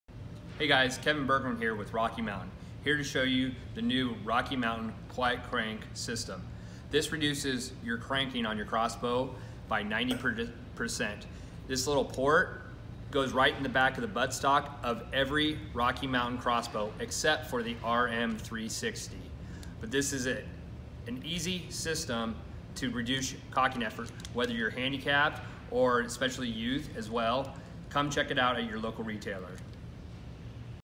Rocky Mountain Quiet Crank
The Rocky Mountain Quiet Crank will fit on any Rocky Mountain Crossbow besides RM360.  This will reduce cocking effort by 90%!  Check it out at your local Rocky Mountain Dealer!